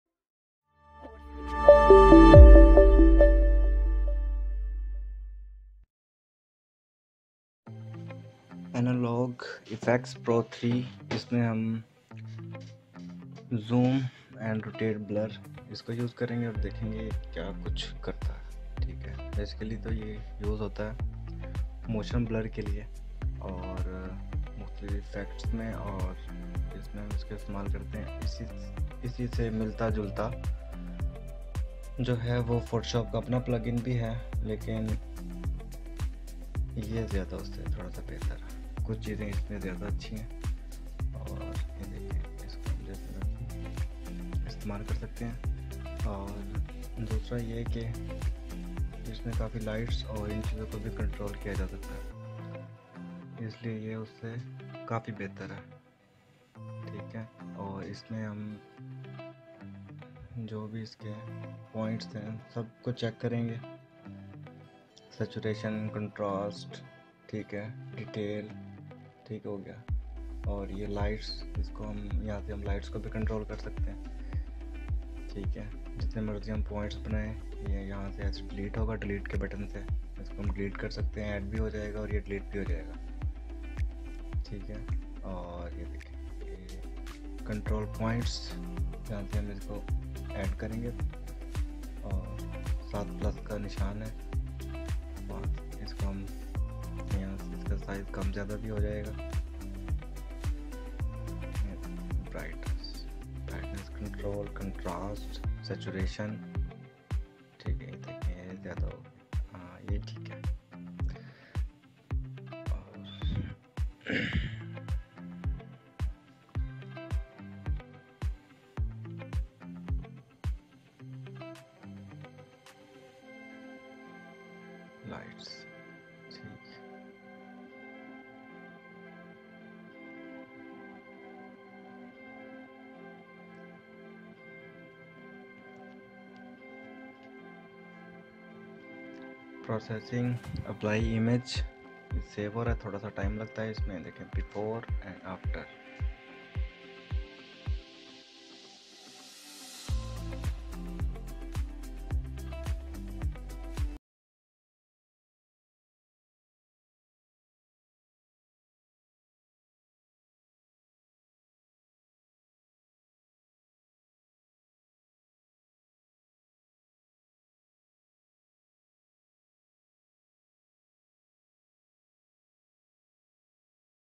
Nik Analog Efex Pro 3 Zoom & Rotate Blur
I really enjoy adding Creative Blur to my images. One of the best and easiest ways of doing this is by using Analog Efex Pro 2, a part of the NIK Collection. In today's tutorial I am working with the Motion Blur Tool and I have some great tips that will help you take advantage of this powerful and creative tool.